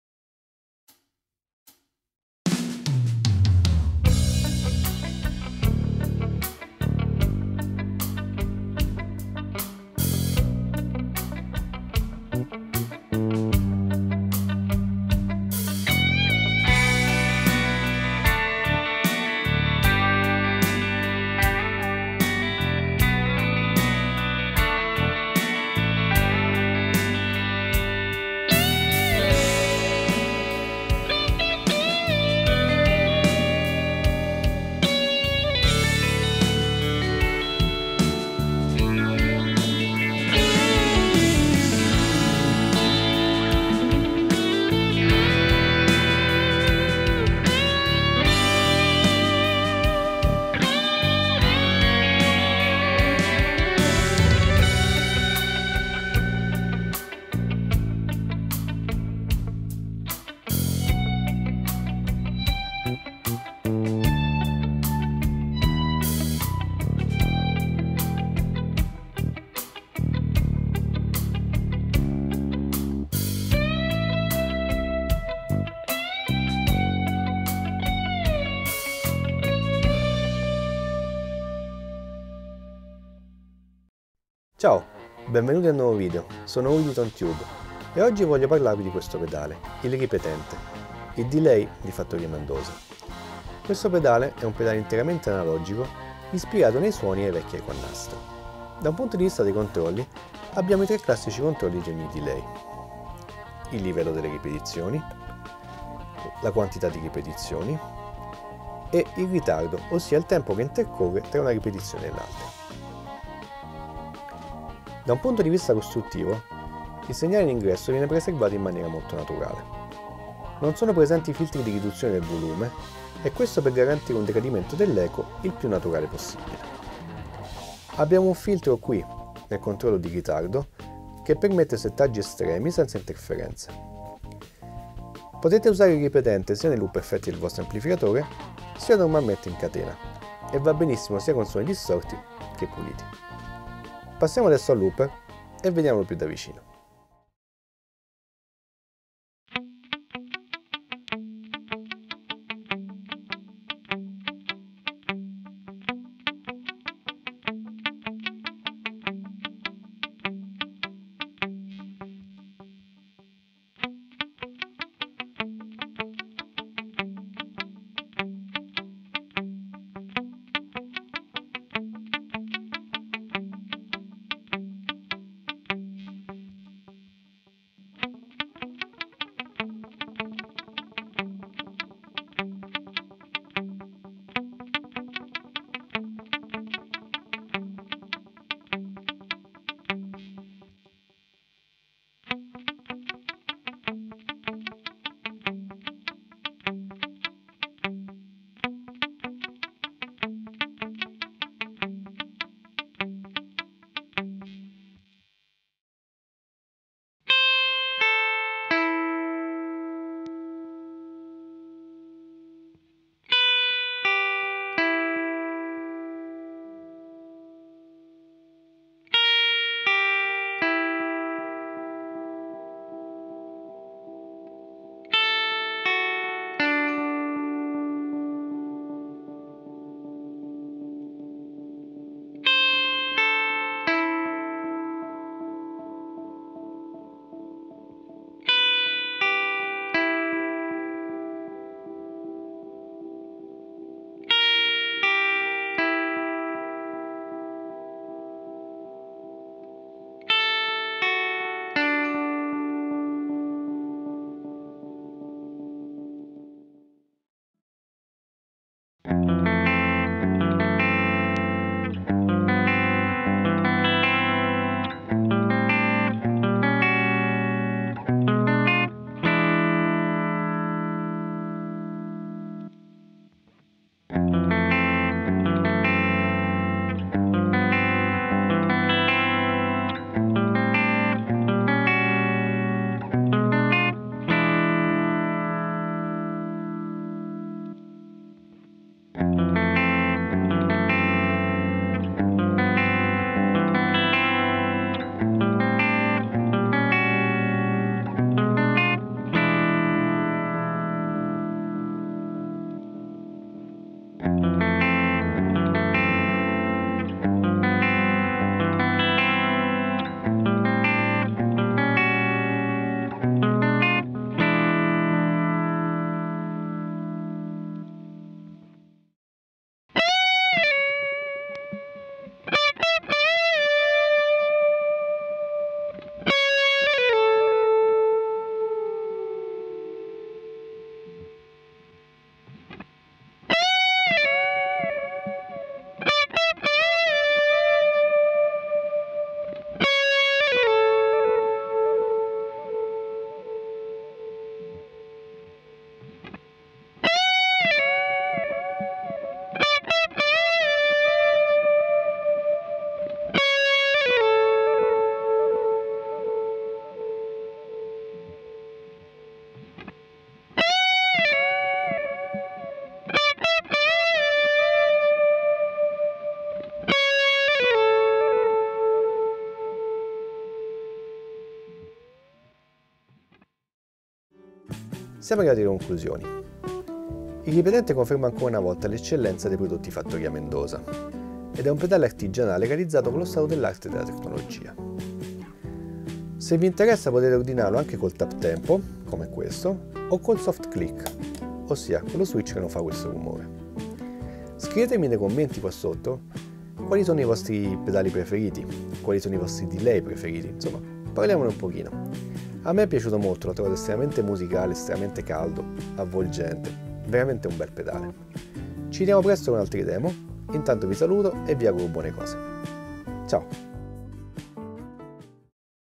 PEDALI PER CHITARRA ELETTRICA | FATTORIA MENDOZA RIPETENTE | DELAY | EFFETTI CHITARRA ELETTRICA
Pedali ed effetti per chitarra elettrica |  pedaliera chitarra | delay analogico

ב"ה

Il Ripetente di Fattoria Mendoza e' un delay analogico artigianale con un carattere marcatamente vintage, che si rifà ai vecchi eco a nastro

Come controlli abbiamo i classici Livello (delle ripetizioni) Ritardo (tempo di delay) e Ripete (feedback o quantita' di ripetizioni). E' possibile impostare il pedale con ripetizioni infinite per costruire interessanti armonie da semplici arpeggi

Il brano nel video e' stato scritto, registrato e mixato da Uri Baranes ed al basso suona Mark Caltagirone